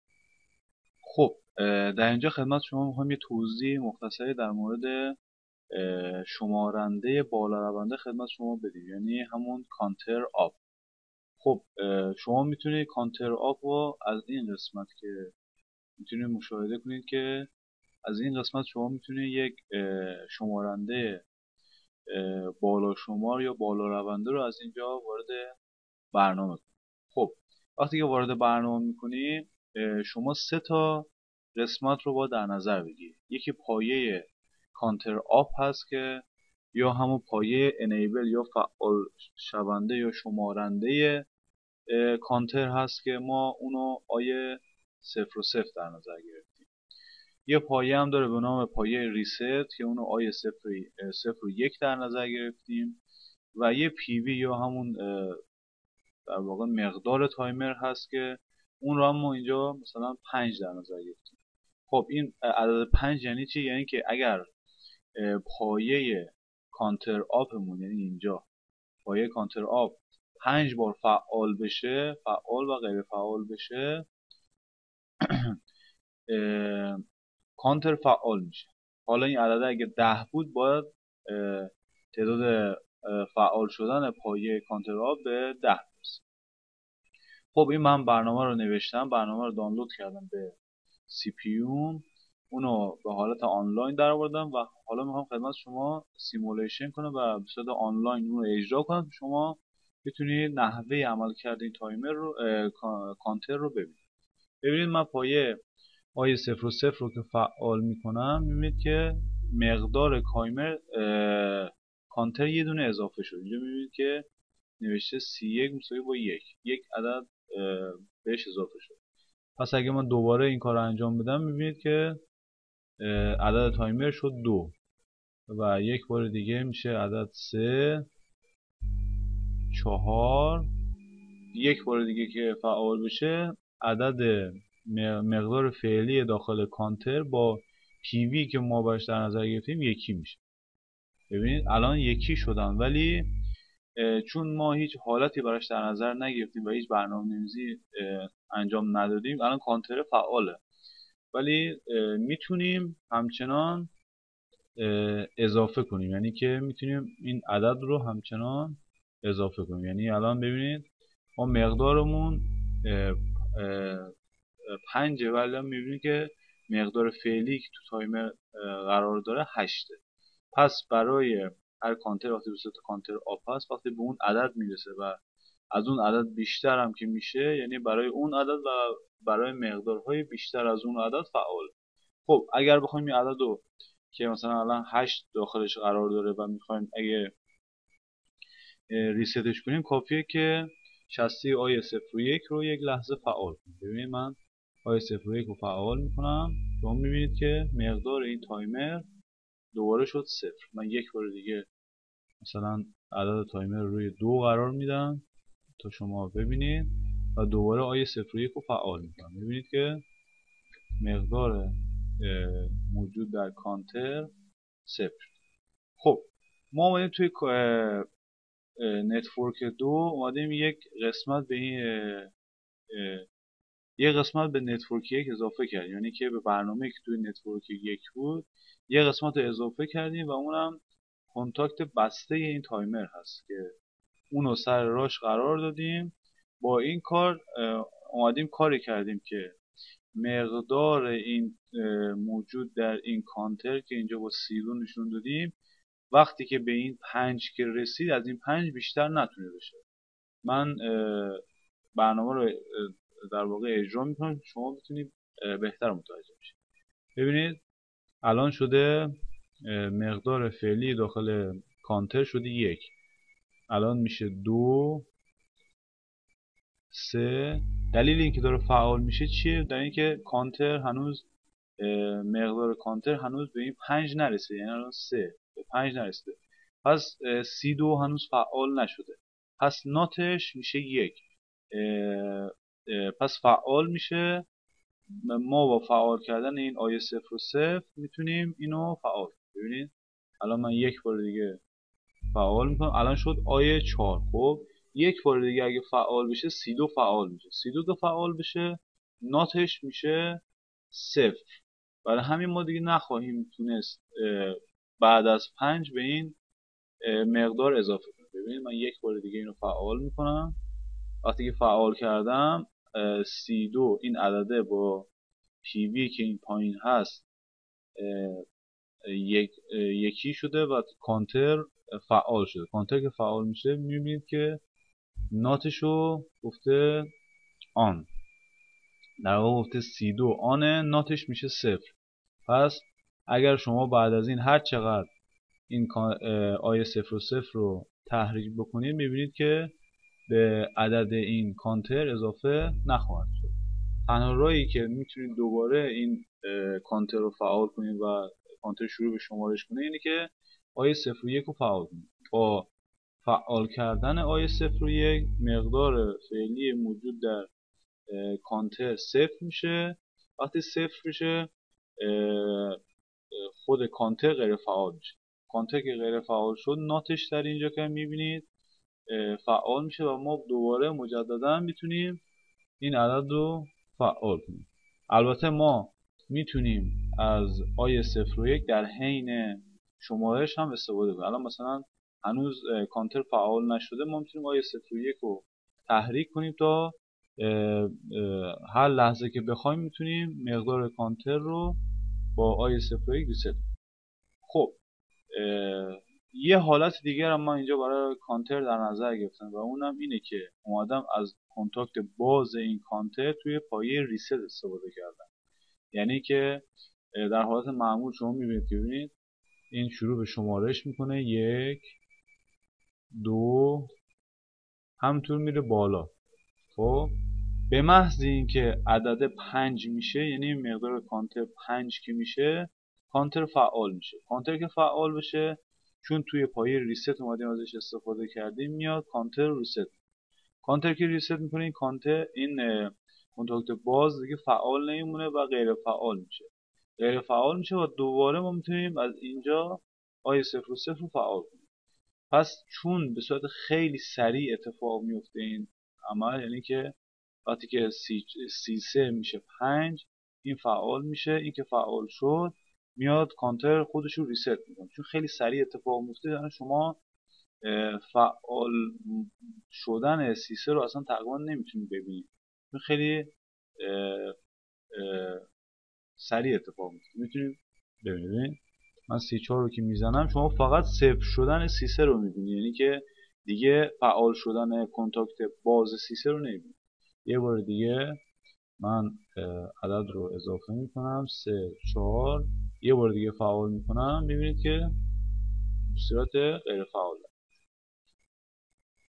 آموزش S7-200 زیمنس ، 8 ، Cunter UP
با استفاده از این شمارنده ، می توانید به صورت صعودی ، عمل شمارش را انجام دهید .
برای دریافت آموزش های رایگان بیشتر به سایت پی ال سی وان مراجعه کنید .

پروژه های عملی انجام شده سورس محور ، همراه با آموزش